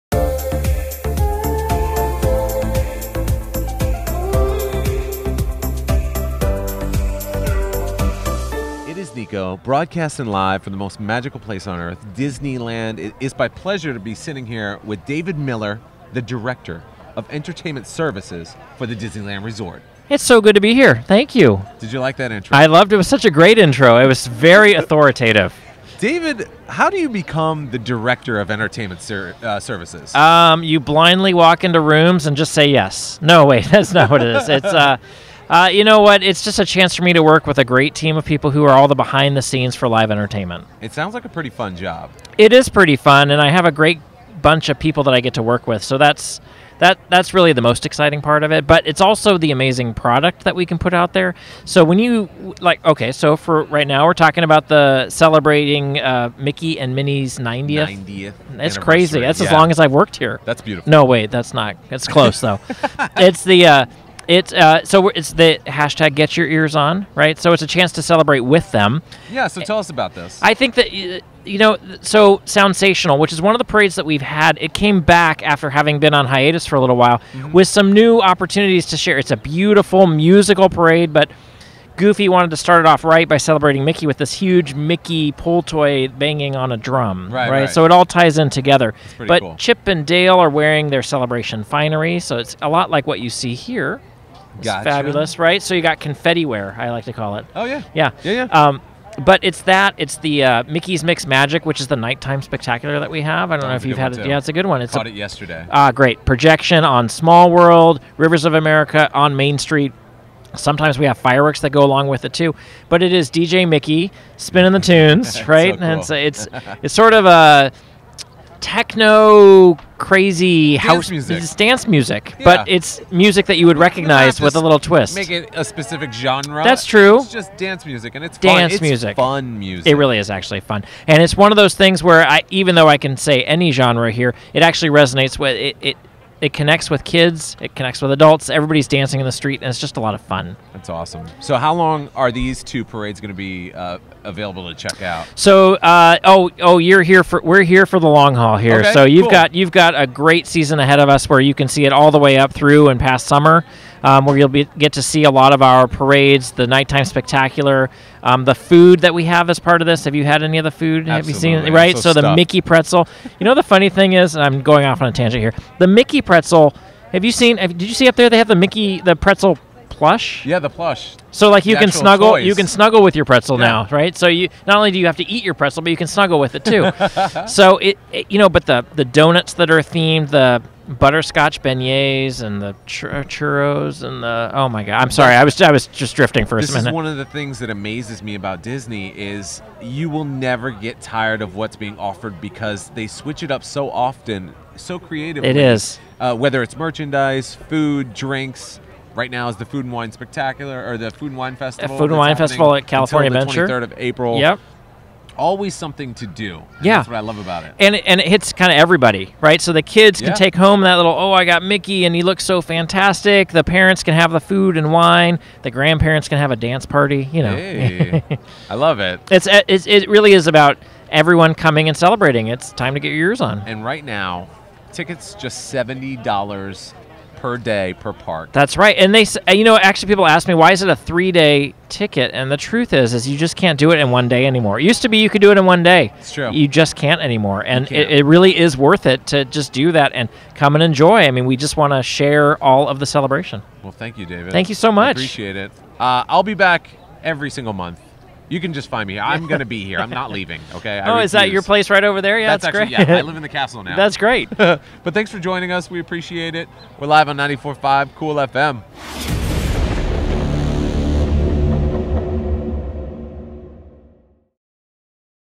Niko with David at Disneyland
Niko and David the Director of Entertainment Services with the Disneyland Resort talk about some of the fun things to expect at Disneyland resorts this season